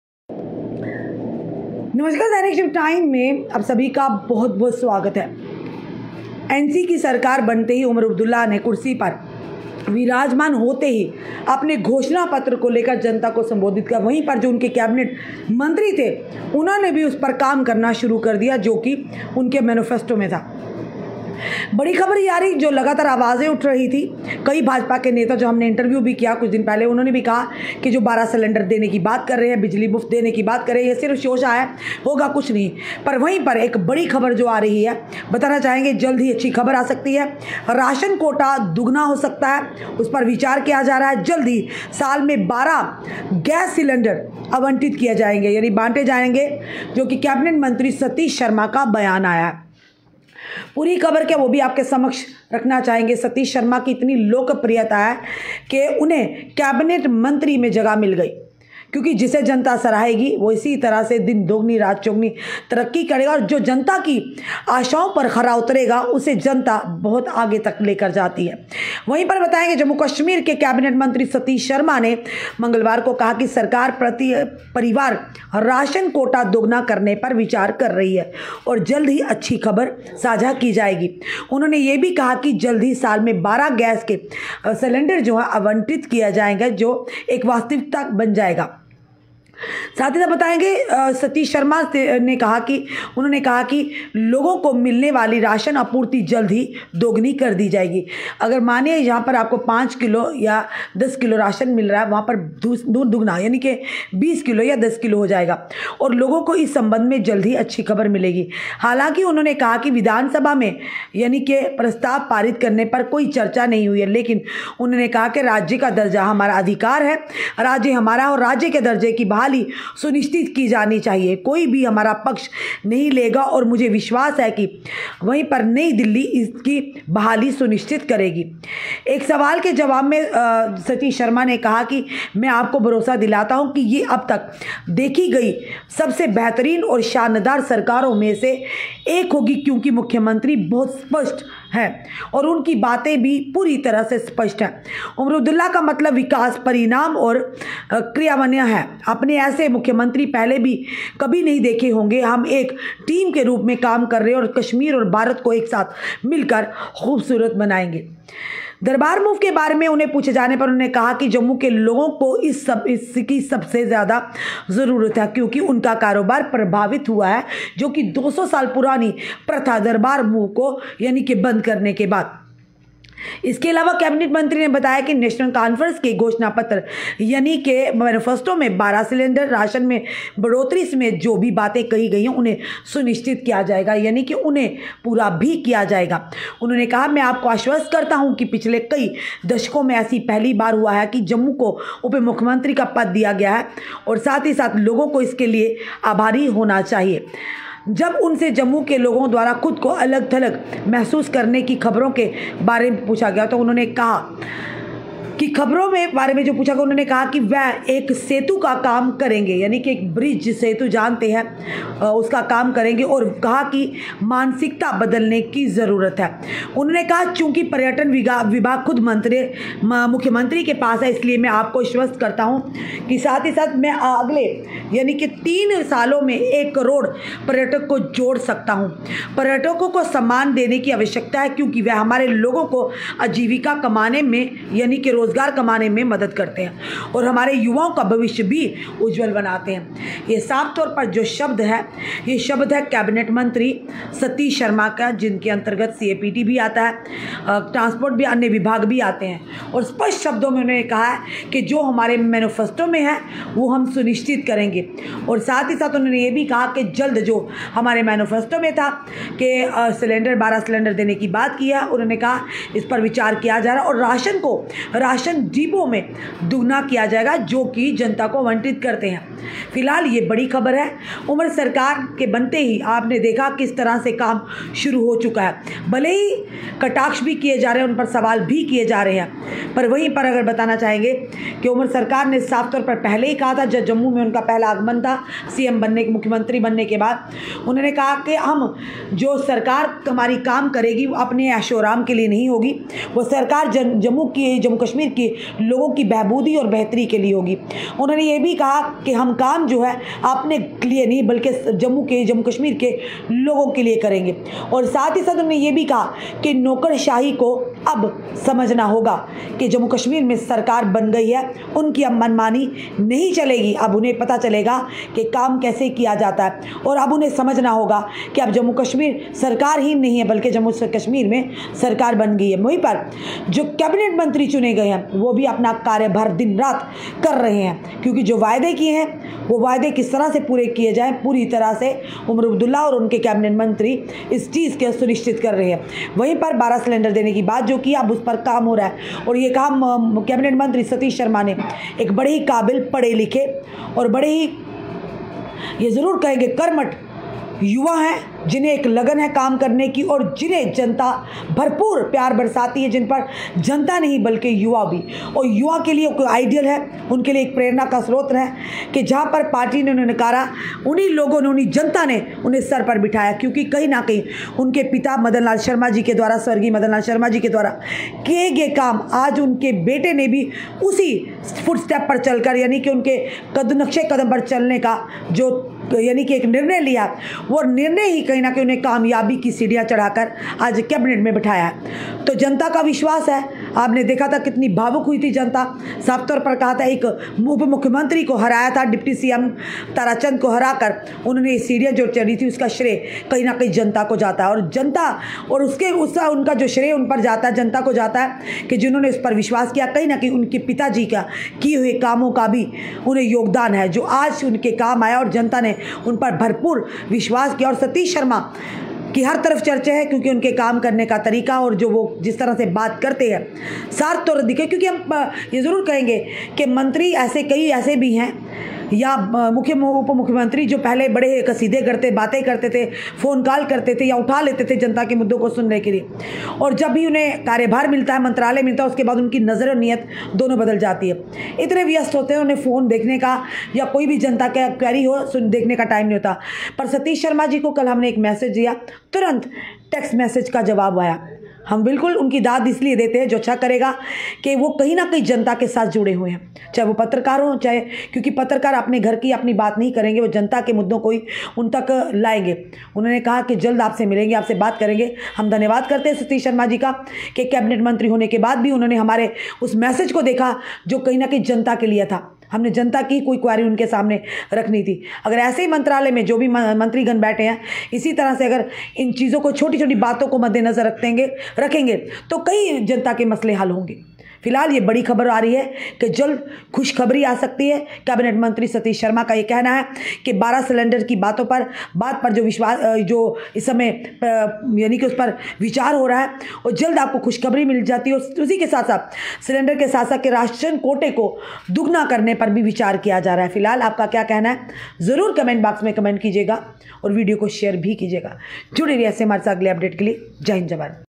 12 gas cylinders and doubling the ration; will give good news soon:
12 gas cylinders and doubling the ration; will give good news soon: Cabinet Minister, Satish Sharma...must watch n share 👇👇👇